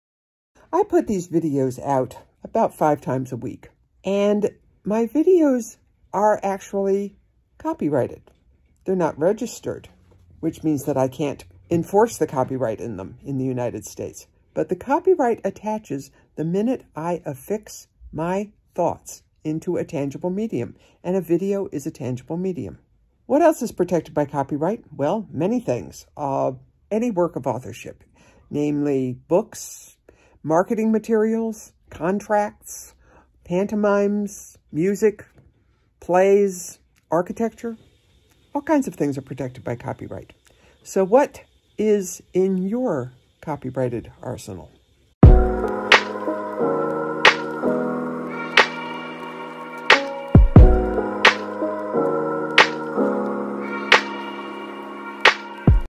Copyright Attaches
Copyright attaches when you affix the work of authorship, whatever that work of authorship may be, into a tangible medium. But you can't do anything about it until you actually register that copyright.